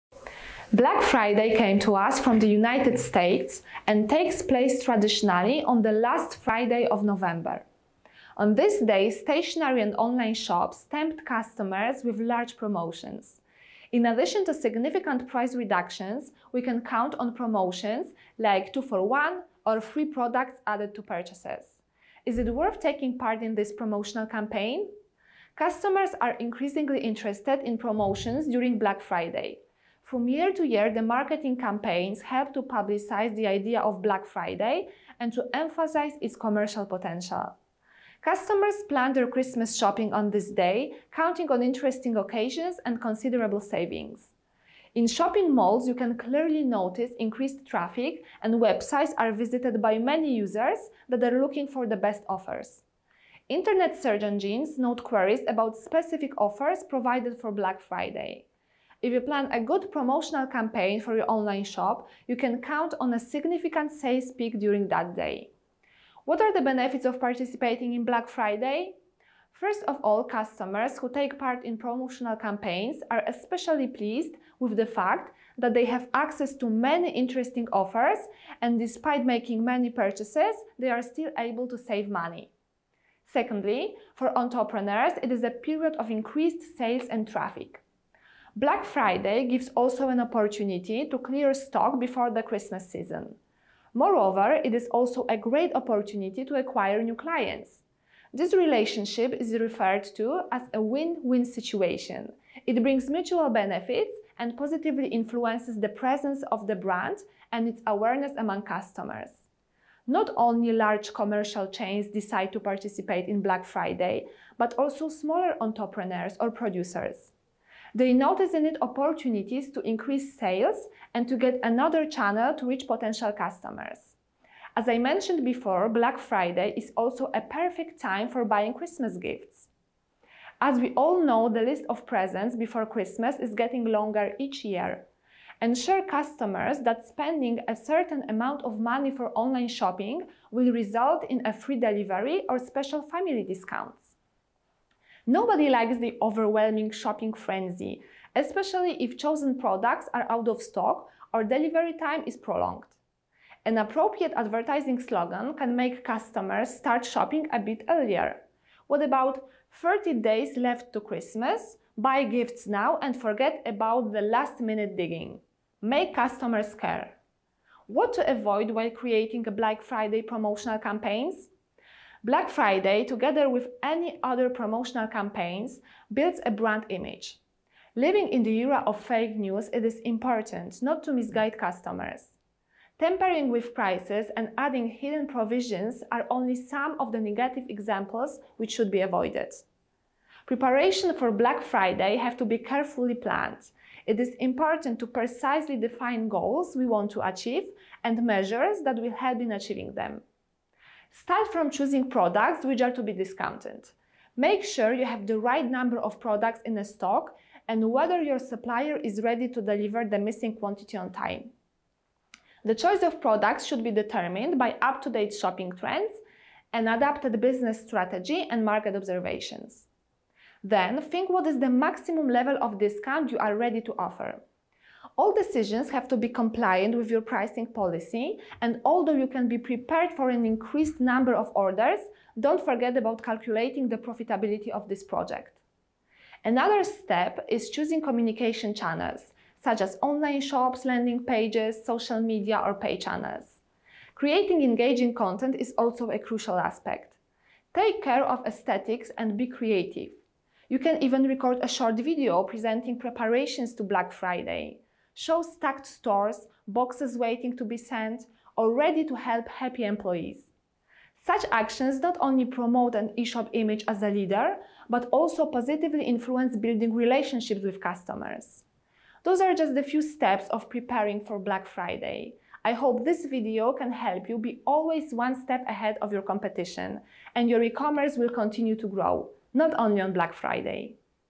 Is it worth to take part in Black Friday?
Is it profitable to take part in Black Friday? What are the benefits of participating in this promotional campaign? Watch the video and find out!